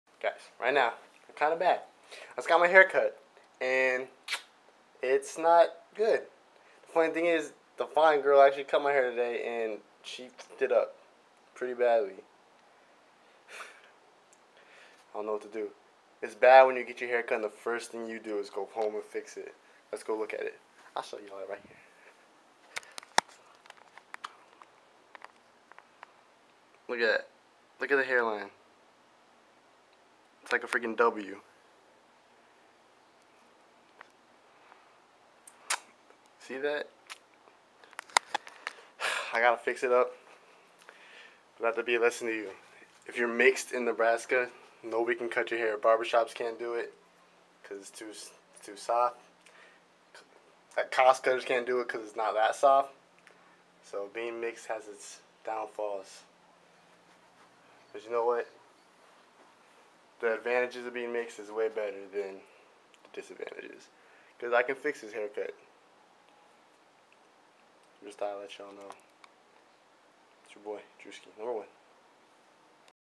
Haircut
Do NOT (I Repeat) DO NOT let a fine gilr cut your hair. Reason- Well you will not be able to focus on letting them know what you want. Another Reason- You will give her more of a tip then she desrves. And another you will be to in awe to ask her to fix something. TAKE MY ADVICE!